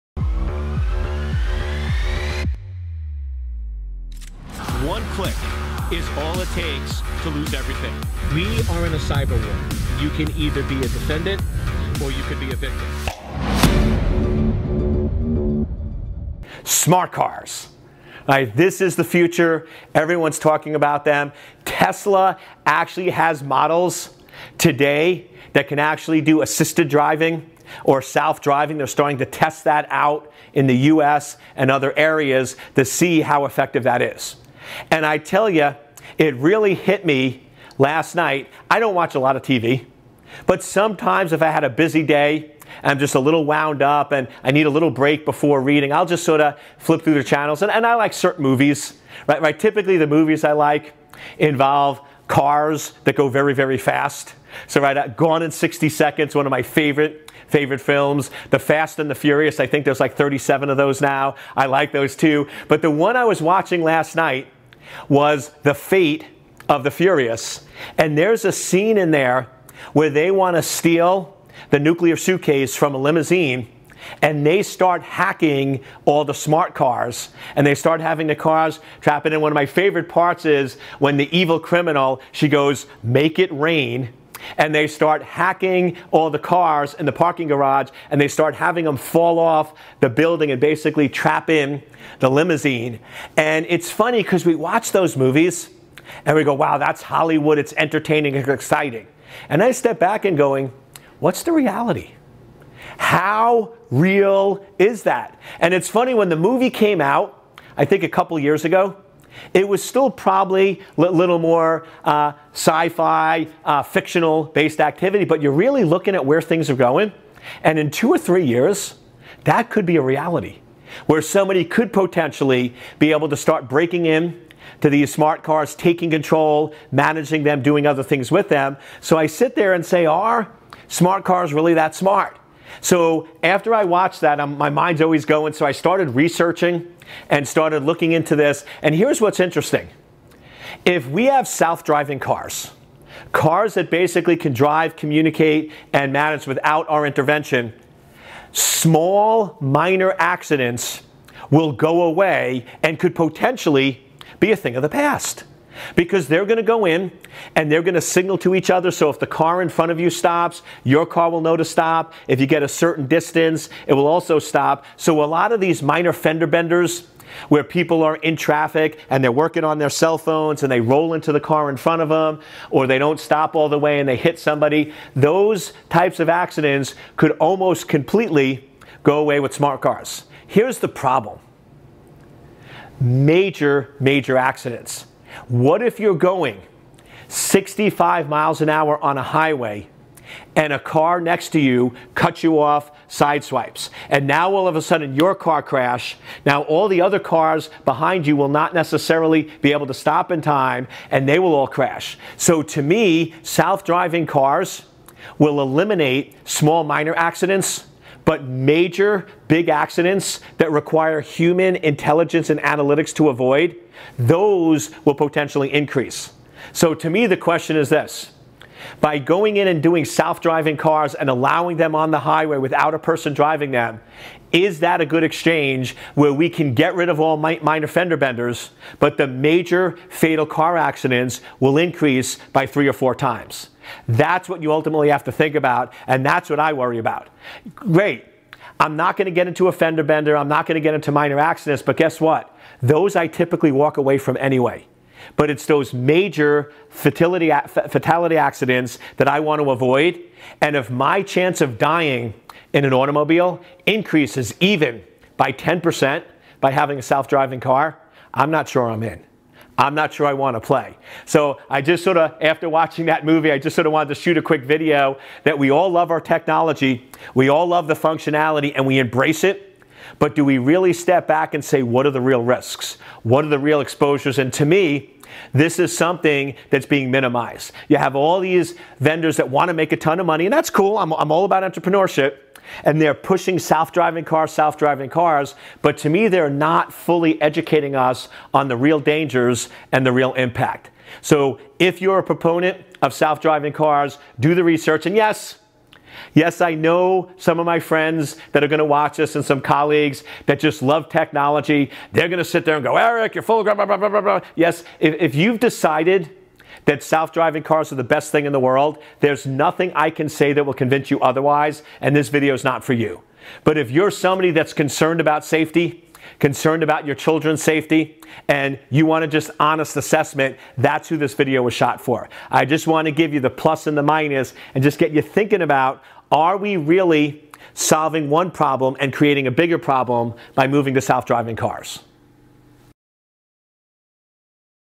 Are Smart Cars The Future? - Dr Eric Cole’s Security Tips
Smart cars used to be a vision of the future. The future is here, but are these smart cars really that smart?